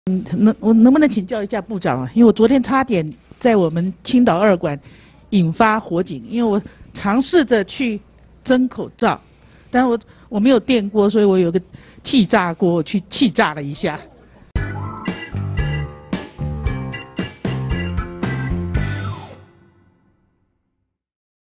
【沃草烏龍球】用氣炸鍋蒸口罩！葉毓蘭自爆差點燒了立法院
用氣炸鍋蒸口罩！葉毓蘭 自爆差點燒了立法院

大家知道立法院昨天差點就燒掉了嗎？中國國民黨排名第二的不分區立委葉毓蘭今(8)天在內政委員會質詢時自爆，她昨天想嘗試蒸 口罩 ，但因為沒有電鍋，就用 氣炸鍋 去蒸，差點引發立法院青島二館的火警。

在4月5日的記者會中，食藥署有特別強調除電鍋外的加熱工具，尚未經實驗證實，如果需要使用則需要控制溫度。氣炸鍋的加熱原理其實與旋風烤箱相同，都是以熱風烘烤物品。指揮中心在會後發出的「蒸口罩注意事項」中，也明確指出不建議使用烤箱。

其實，就連用電鍋蒸口罩，也必須將口罩放在內鍋中，並用內鍋架隔開內鍋與電鍋壁，才能讓口罩不會過熱變形。大家在蒸口罩的同時，也要留意溫度，注意安全呦！

🔎 中國國民黨立委葉毓蘭怒批一週兩片口罩制：內褲一禮拜能只穿兩天嗎？

【蒸口罩注意事項】：

1.用家裡原本的電鍋即可，還是要買全新的電鍋?
答：家中既有之電鍋即可進行口罩之乾熱滅菌，用完後以食品用清潔劑適度清洗後晾乾即可繼續使用，不一定要買新的。

2.蒸完口罩還可以用來蒸煮食品嗎?
答：同上題，以食品用清潔劑適度清洗後晾乾即可繼續使用。

3.口罩氣味重怎麼處理?
答：若感覺口罩有異味、髒汙或破損，均建議立即更換。

4. 用烤箱行不行?
答：食藥署未進行經烤箱處理口罩之殺菌能力及過濾效率評估，且一般烤箱可達到之溫度較電鍋乾熱高很多，可能破壞口罩結構，不建議使用。

5.用負離子吹風機行不行?
答：食藥署未進行以負離子吹風機處理口罩之殺菌能力及過濾效率評估，惟吹風機可能使口罩上附著之病原體透過氣流懸浮於 空氣中，不建議使用。

6.用煮的行不行？
答：口罩內襯帶靜電之不織布為產生防護效能之關鍵，以蒸氣或其他溼式的殺菌辦法都可能破壞其結構，造成防護效能下降，不建議使用。

7.暫時拿下來如何收納？
答：脫下之口罩，建議將口罩污染一面往內摺後，放置於乾淨、透氣的容器內(如紙袋等)保存，用於保存之容器，應於使用後丟棄或定期清潔;另外，脫除口罩時，應避免直接碰觸口罩外側，以避免其上污染病菌透過手的觸摸而散佈，口罩脫除後也應執行手部衛生。

8. 可以用「消毒用帶殺菌燈的小型烘碗機」嗎？
答：食藥署未進行以殺菌燈處理口罩之殺菌能力及過濾效率評估，不建議使用。